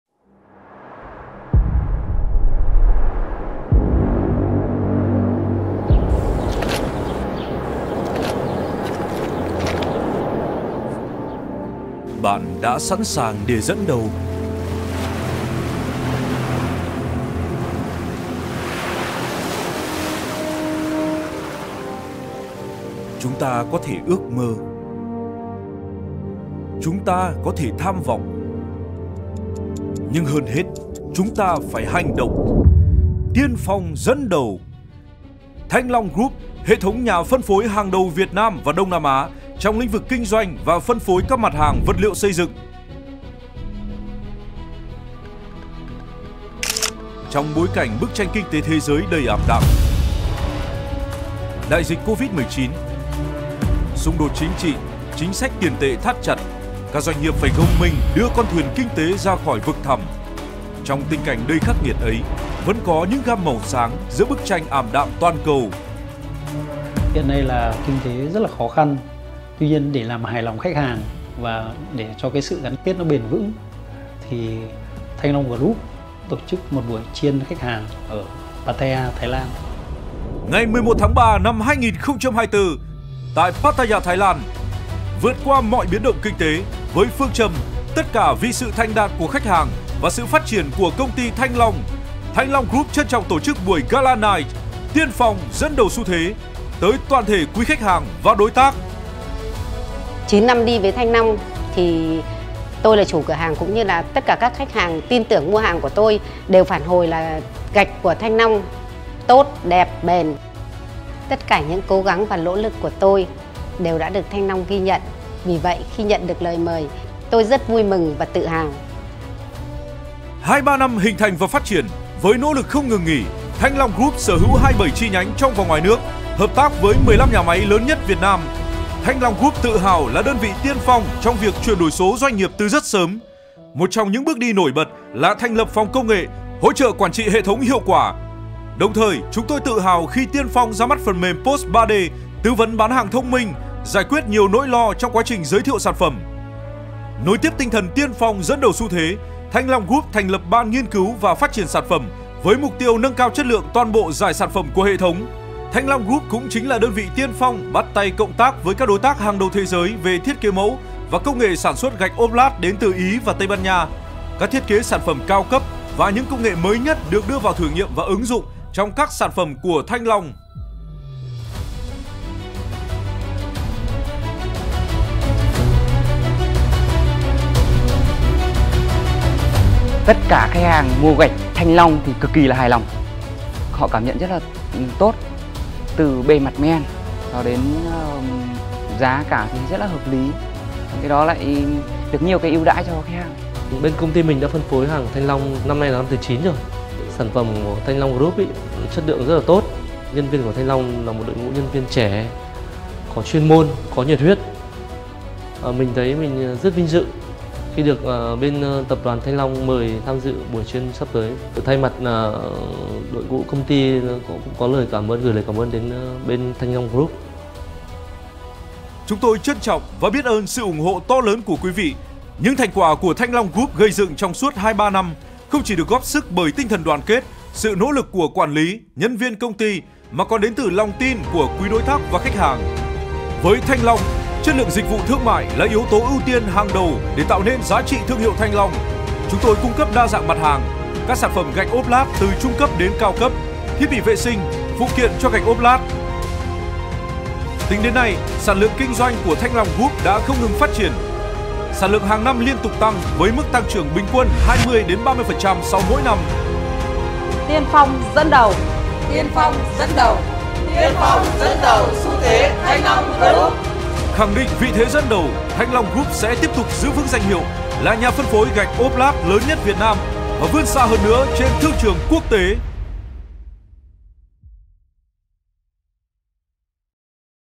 Tiên phong dẫn đầu | Phim giới thiệu doanh nghiệp Thanh Long Group
Dự án sản xuất phim giới thiệu doanh nghiệp Thanh Long Group - Hệ thống nhà phân phối hàng đầu Việt Nam trong lĩnh vực kinh doanh và phân phối các mặt hàng vật liệu xây dựng, được thực hiện bởi ShopVideo by ColorStudio - Đơn vị chuyên sản xuất video Marketing dành cho doanh nghiệp vừa và nhỏ.

📌 Địa chỉ: 95 K2 Nguyễn Văn Giáp, Phú Đô, Mỹ Đình, Hà Nội